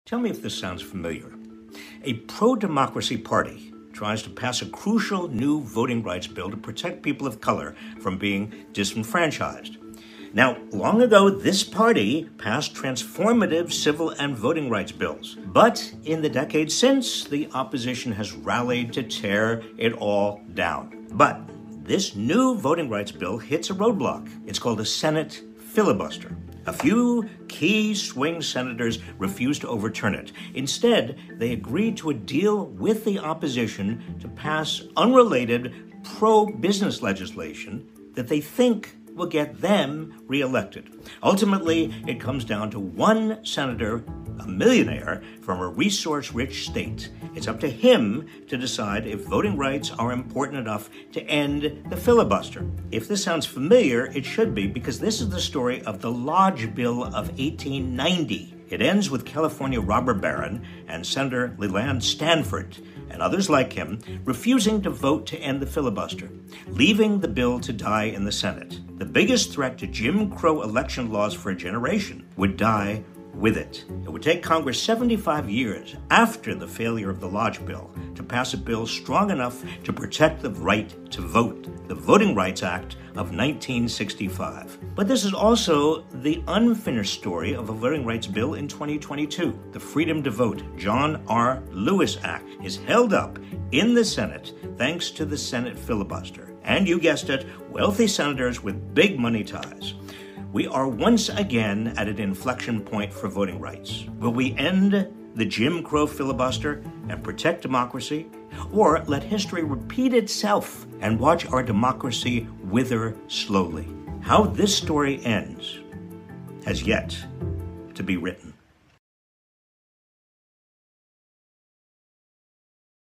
News and NOTES 📝
Robert Reich talks filibuster and history of it...